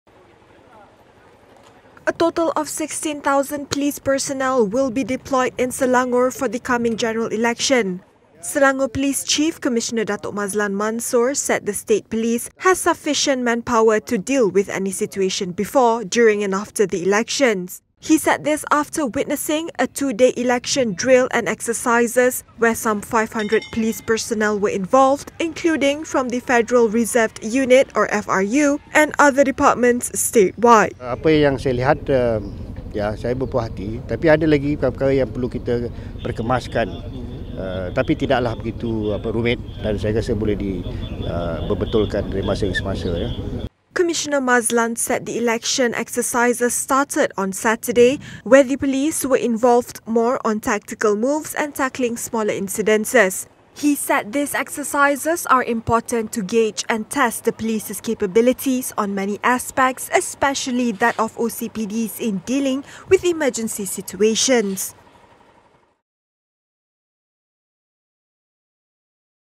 16,000 police personnel to be deployed in Selangor during GE14
Selangor police chief Comm Datuk Mazlan Mansor said a total of 16,000 police personnel will be deployed in Selangor for the coming general election.

Comm Mazlan said this after witnessing a two-day election election drill and exercises where some 500 police personnel were involved, including from the Federal Reserved Unit (FRU), and other departments statewide.